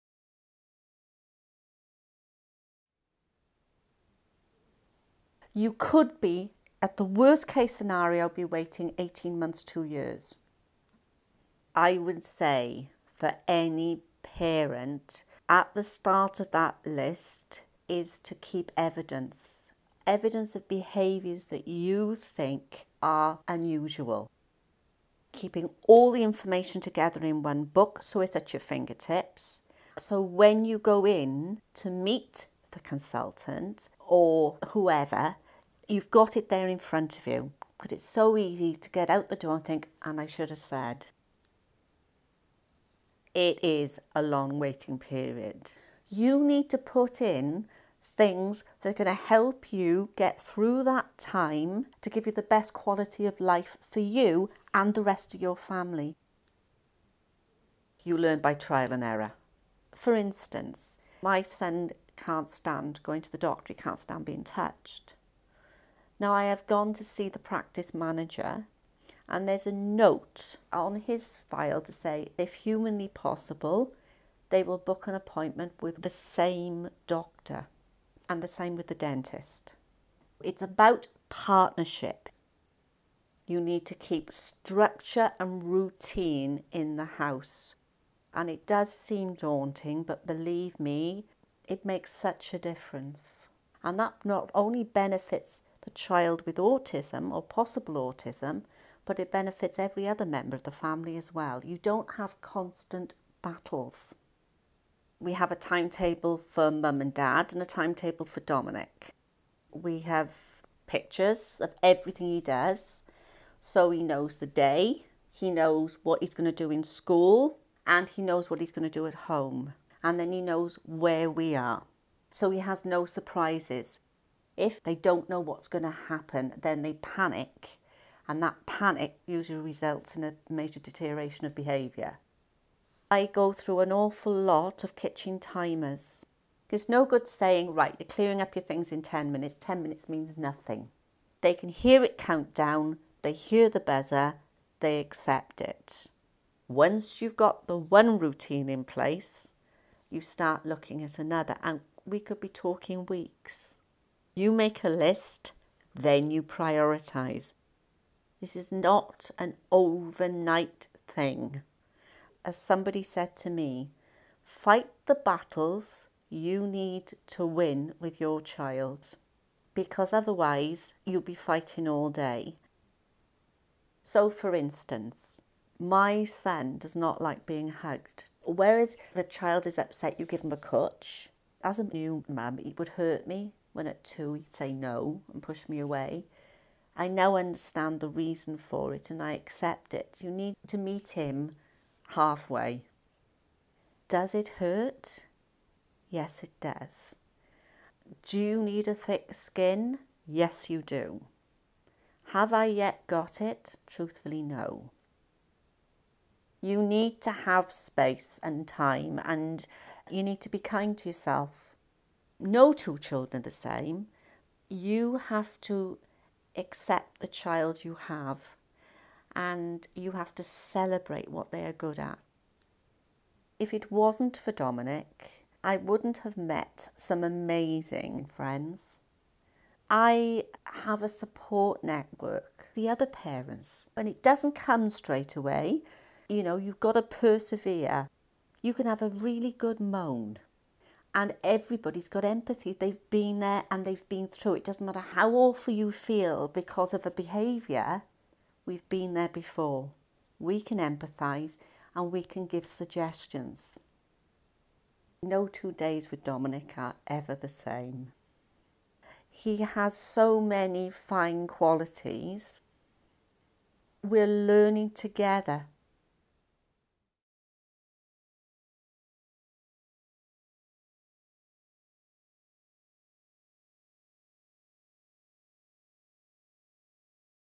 Autism - A Parent's Perspective
A pre assessment story informing and supporting families and carers of children awaiting an autism assessment. This was developed by a group of parents and young people based on their experiences of the referral process and describes strategies that can be tried whilst families are waiting for a referral for an assessment.